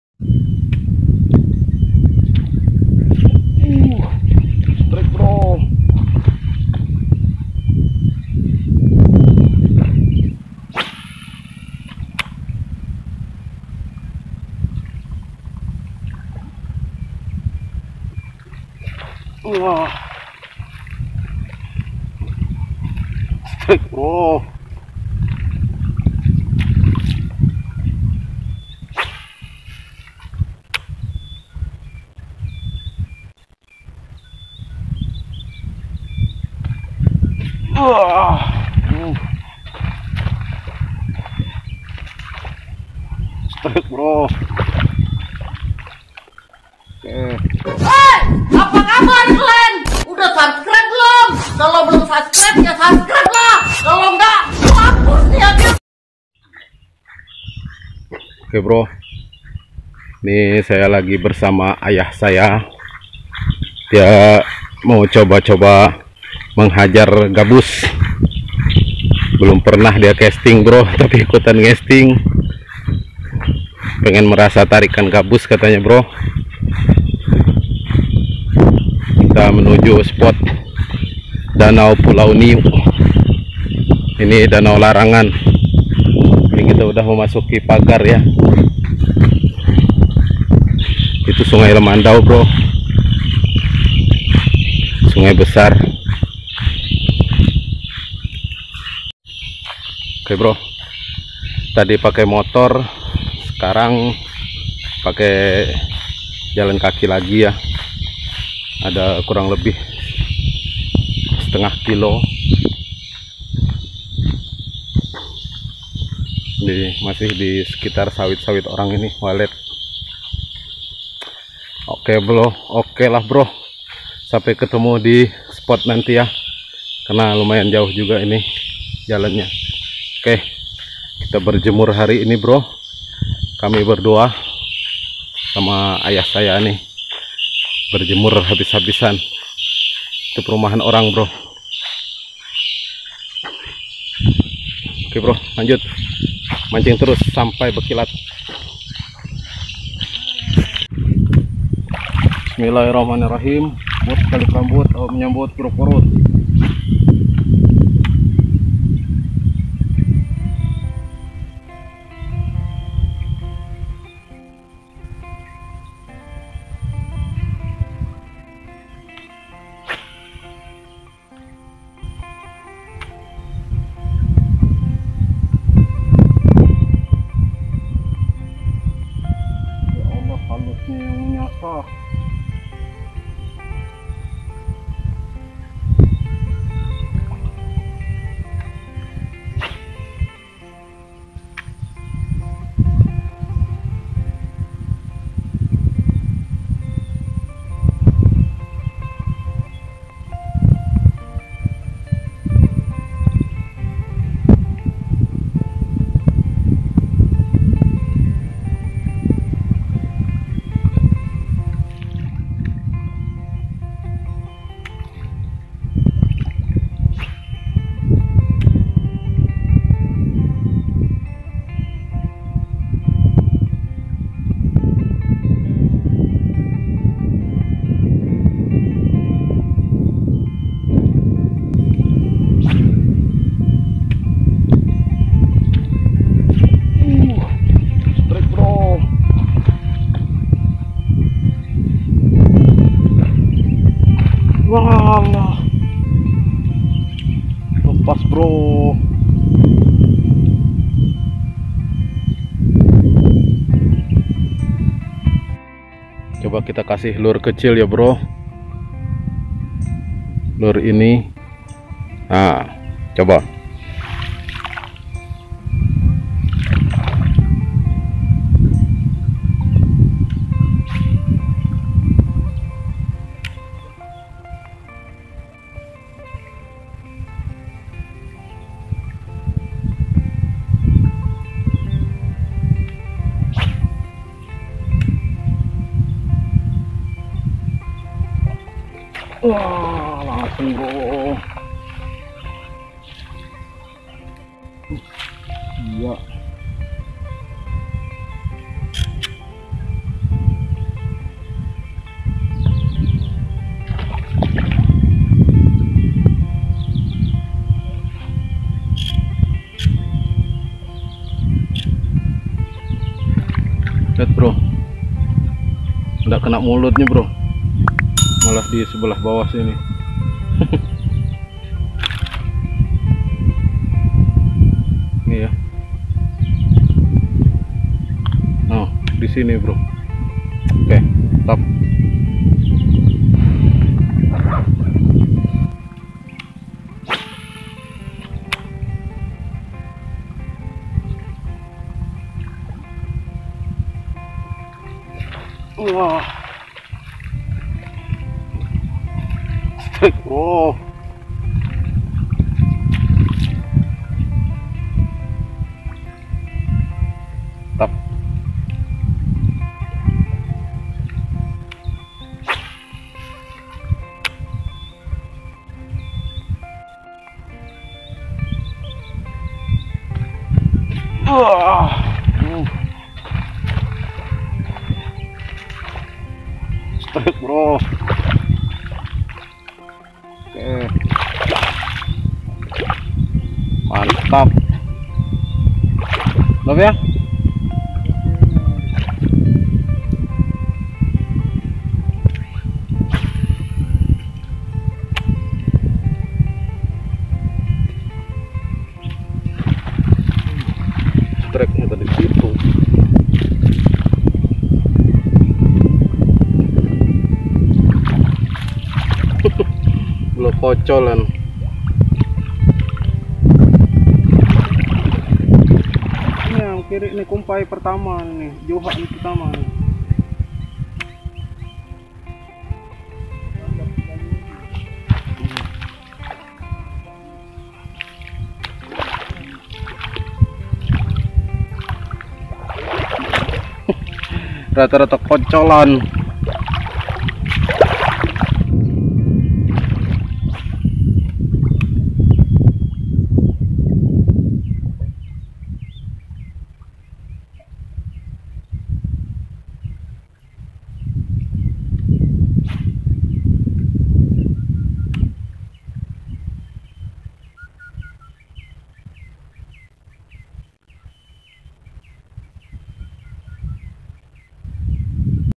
Casting gabus di danau larangan bikin ketagihan
assalamu'alaikum kawan semua.

kali ini yansah pancing casting gabus lagi di danau larangan yang bikin ketagihan.

gabus disini tinggal kocolan jadi harus pakai lure kecil seperti yg di video berikut ini...

semoga terhibur

mancing terus sampai bakilat.